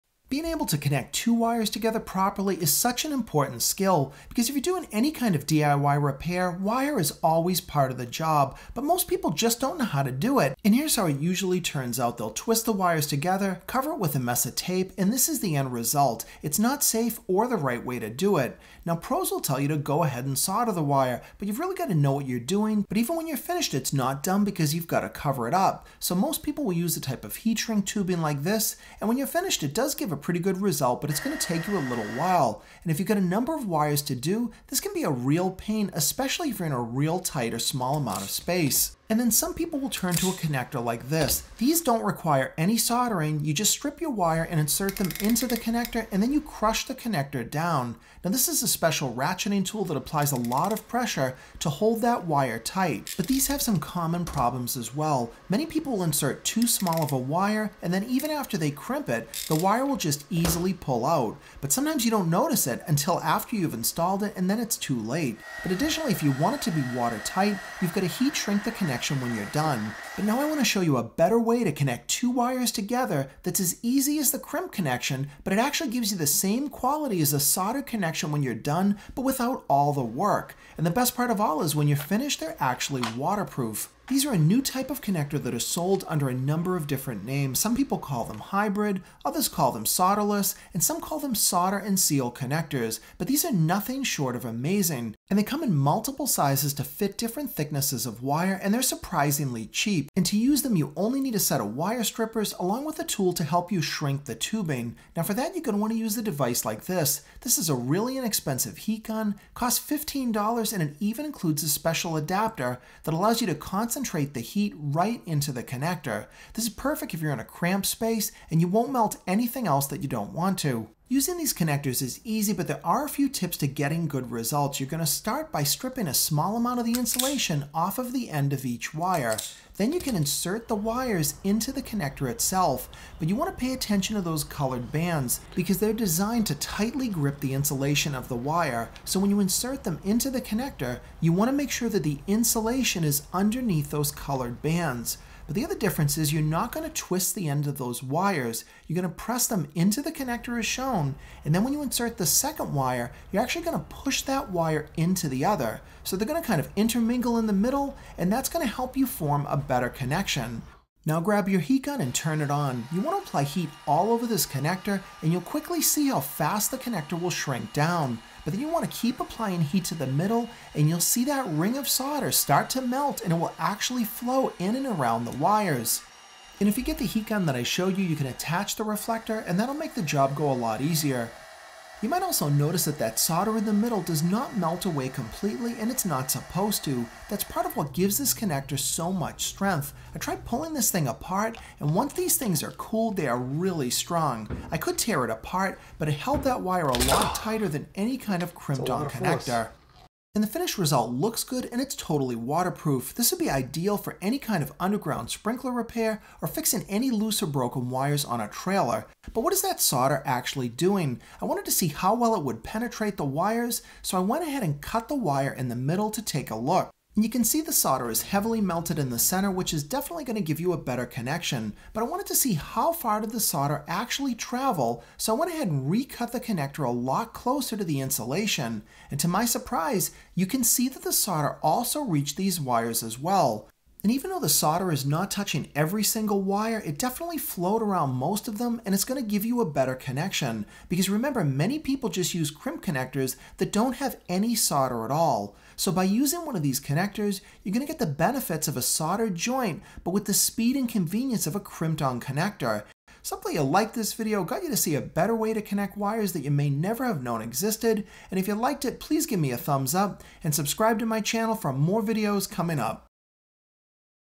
Never Solder Again - How to Connect Wires the EASY Way
Connecting wire can be a real pain, connections fall apart, break or just don't work. Learn how to connect & splice wires the EASY way that's fast, cheap & surprisingly easy for anyone.

Heat Gun